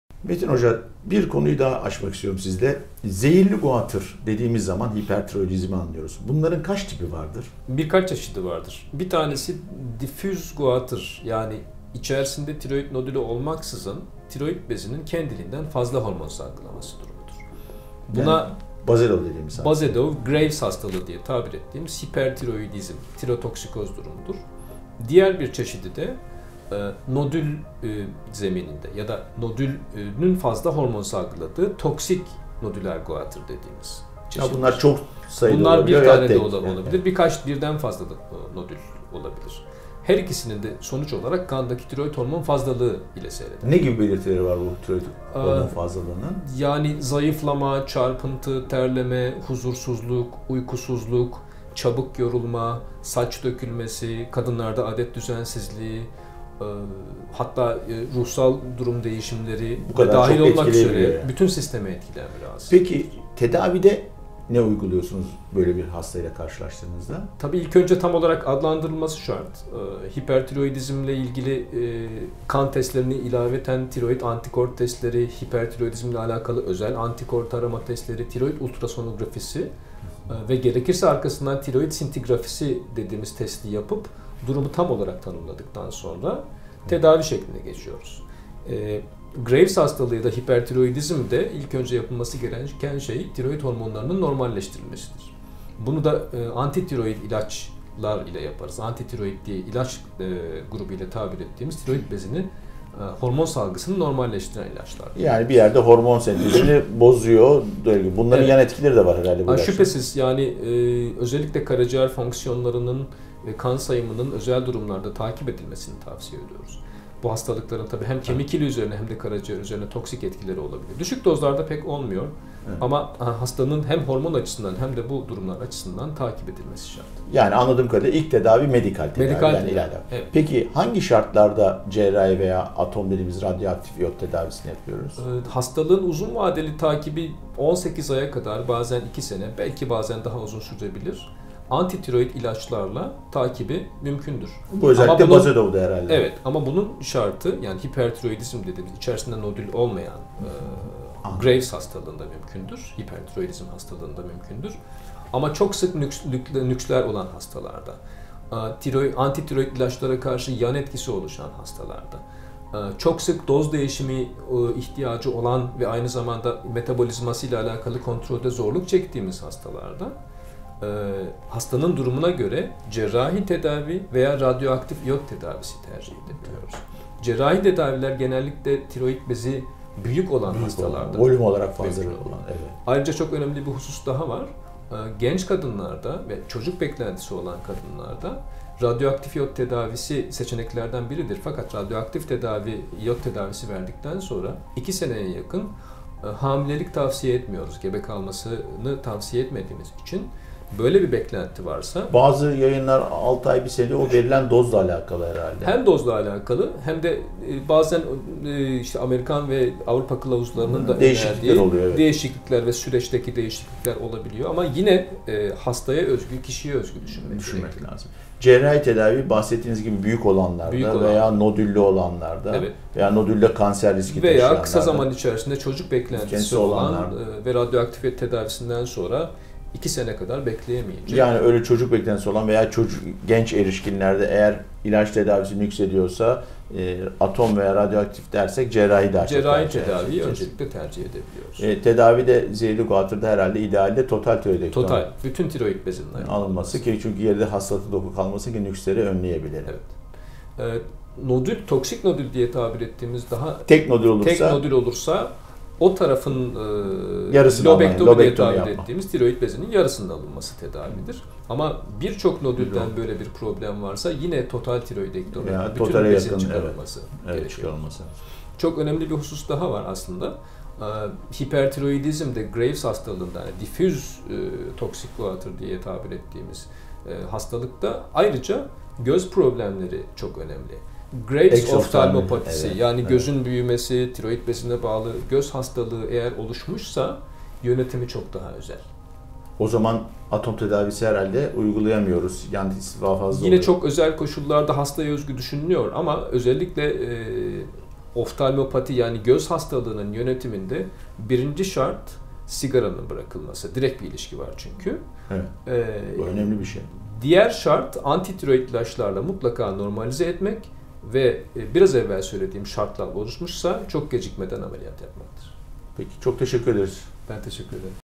Prof. Dr. Serdar Tezelman & Dr. Metin Alış - Zehirli Guatr
Prof. Dr. Serdar Tezelman ve Dr. Metin Alış - Zehirli Guatr hakkında bilgi veriyor.